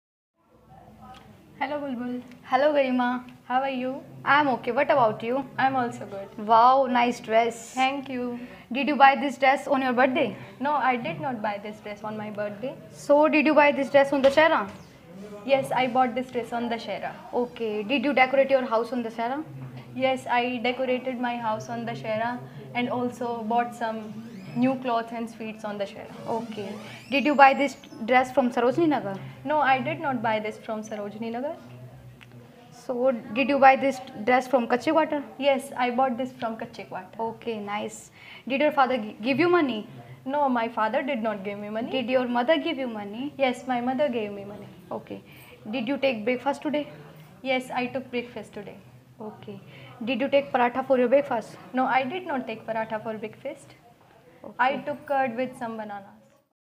A random conversation between students
a role play played between two students of American institute.